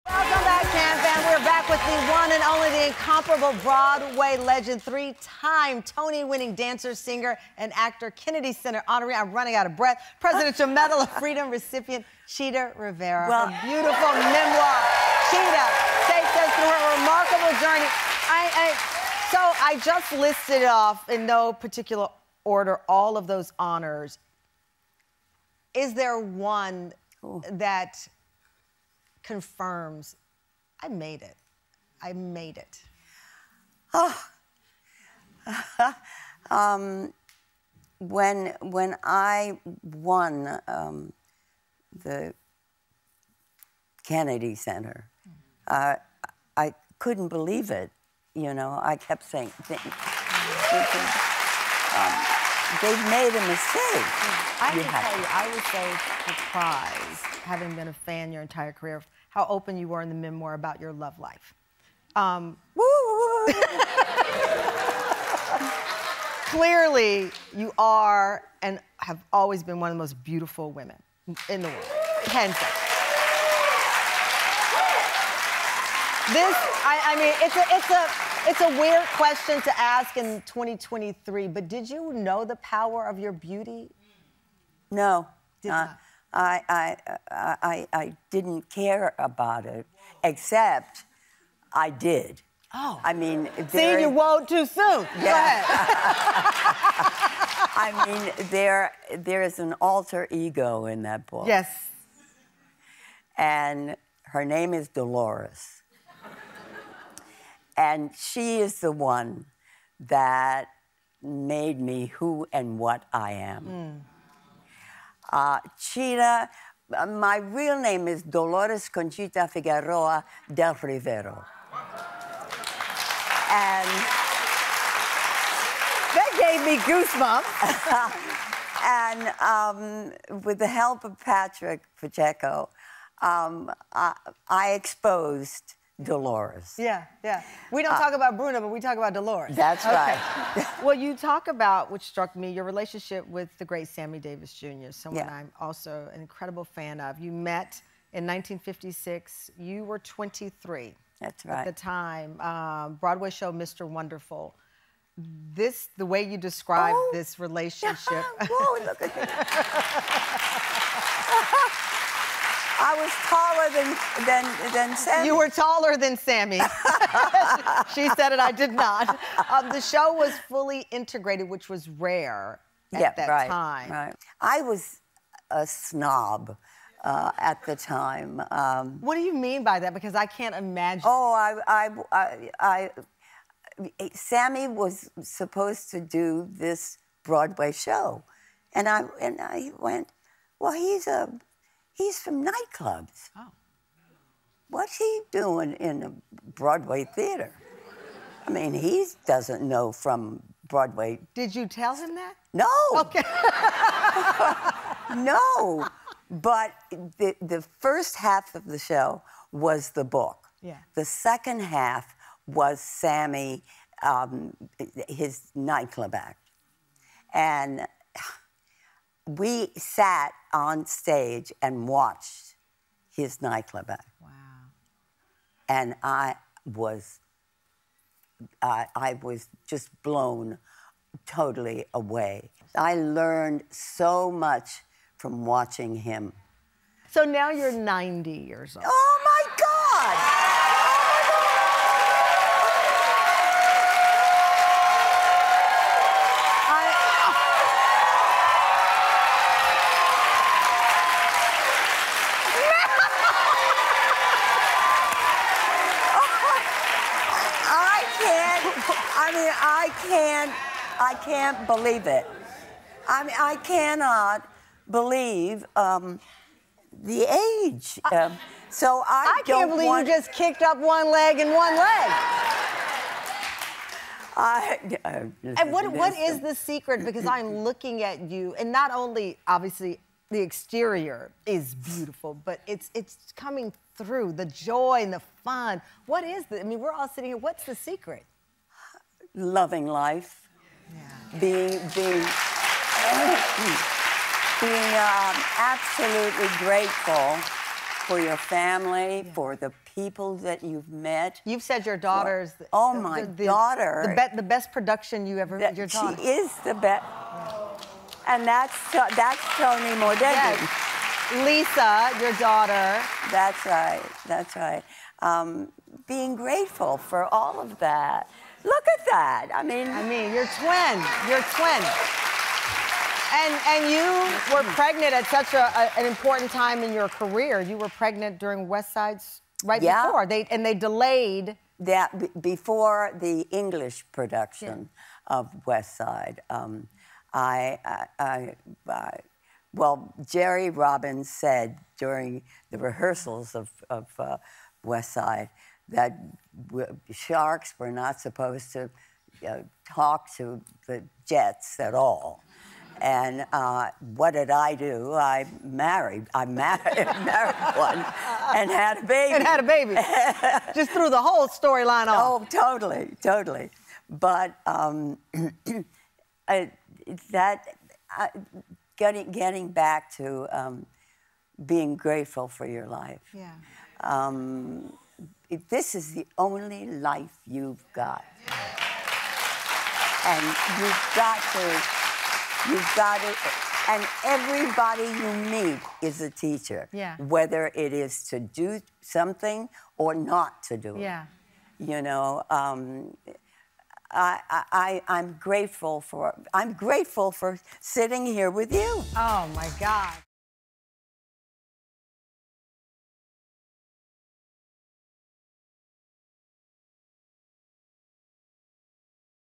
At 90, Chita Rivera Says the Secret to Life Is Being Grateful
Broadway legend Chita Rivera joins us in-studio to talk about her groundbreaking career that includes roles in “West Side Story,” “Chicago” and more. From becoming the first Latina to receive a Kennedy Center Honor to her wind whirl romance with Sammy Davis Jr.—Chita Rivera bares all in this sit-down discussing her self-titled memoir. This icon may be 90 years old, but her spirit is more youthful than ever!  Don’t miss our sit-down with the one and only Chita Rivera.